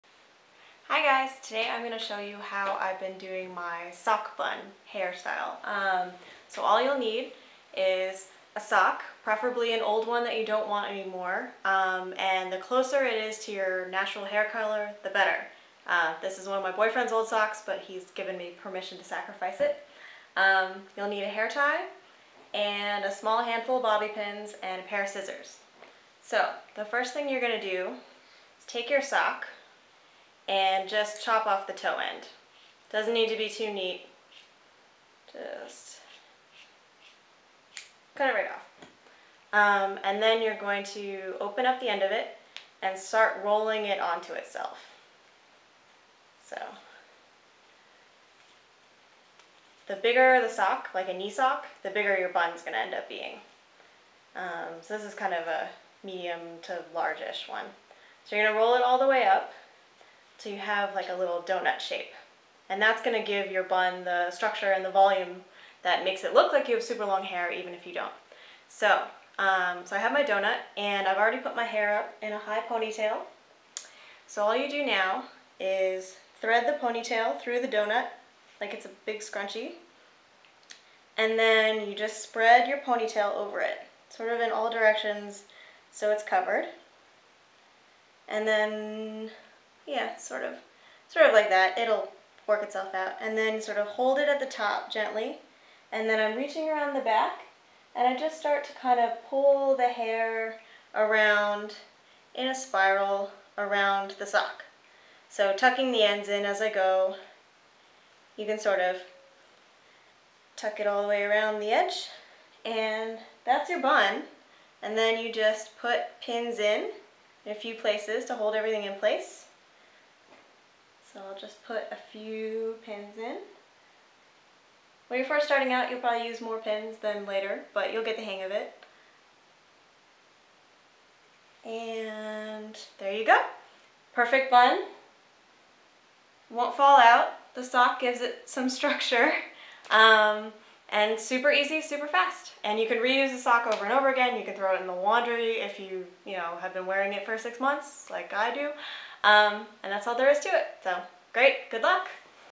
the perfect sock bun
A quick tutorial for my personal style blog on how to style your hair into a perfect bun using a sock.